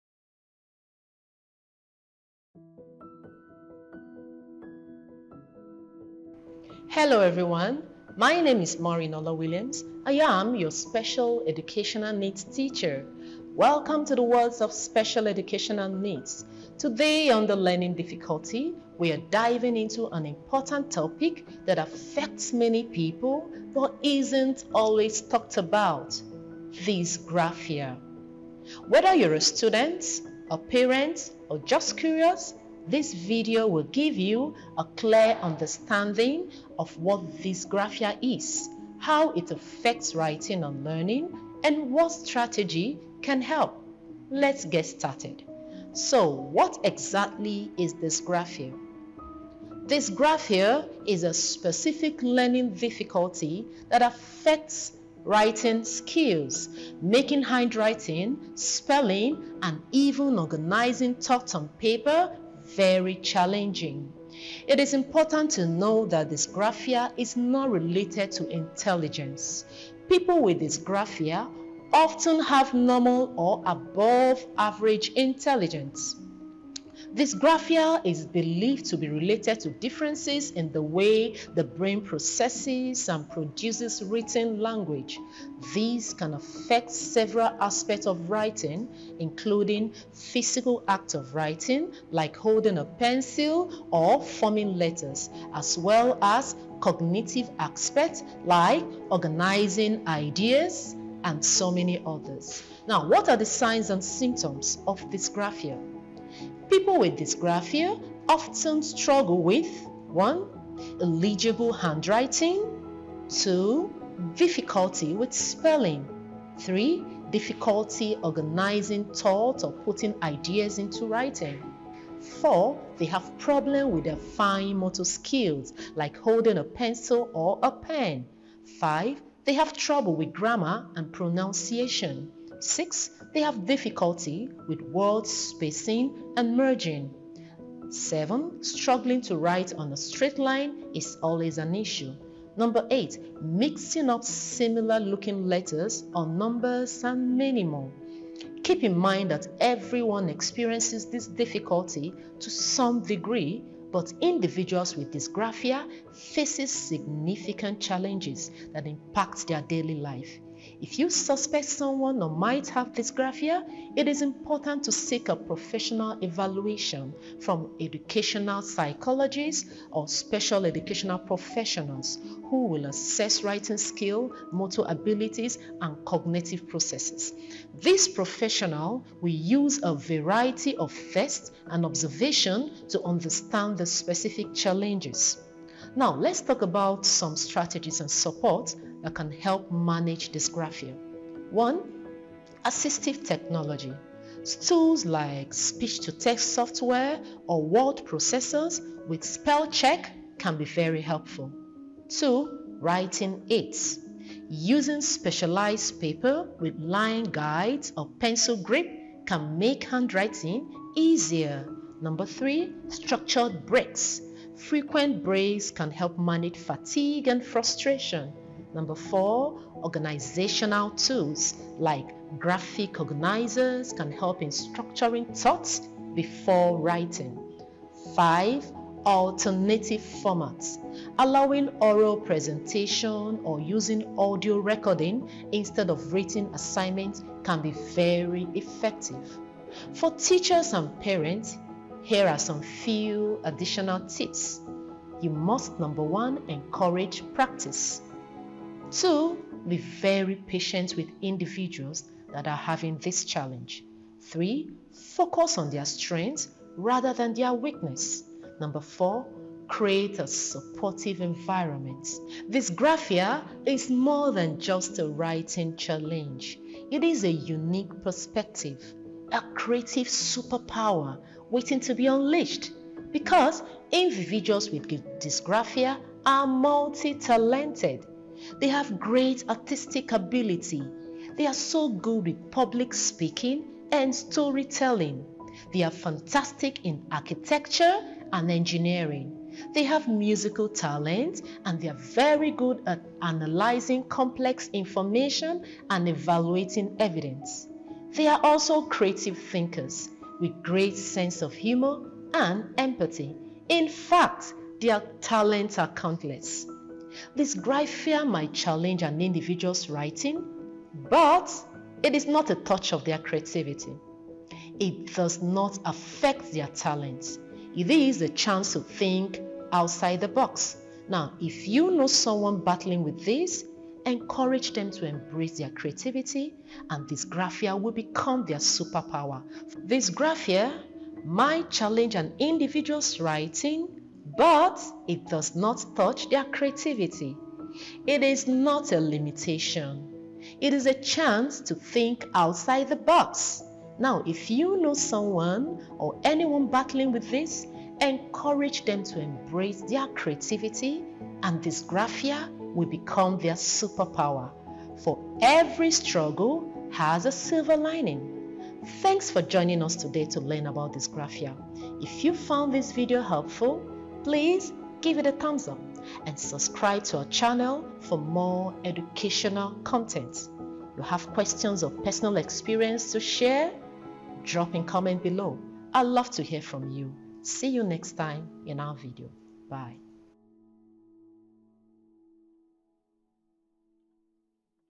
WHAT YOU MUST KNOW ABOUT SPECIAL EDUCATION (DYSGRAPHIA) PART FIVE BY MAUREEN OLA-WILLIAMS
MAUREEN OLA-WILLIAMS TALKS ABOUT SPECIAL EDUCATION. (SEN) FOCUSING ON DYSGRAPHIA. MAUREEN OLA-WILLIAMS IS A DIRECTOR AT LYCEUM MONTESSORI SCHOOL AND LYCEUM COLLEGE ABEOKUTA OGUN STATE. HOLDS A BSC. IN BIOCHEMSTRY, PGDE, DIPLOMA IN MOTESSORI EDUCATION, MASTERS IN EDUCATIONAL SCIENCE AND PHD IN VIEW IN EDUCATIONAL MANAGEMENT. SHES A MEMBER OF INTERNATIONAL DYSLEXIA ASSOCIATION (IDA) IN BALTIMORE USA , NATIONAL ASSOCIATION OF SPECIAL EDUCATION TEACHERS (NASET) BASED IN NEW YORK USA AND INTERNATIONAL ASSOCIATION FOR SPECIAL EDUCATION (IASE) BASED VIRGINIA USA.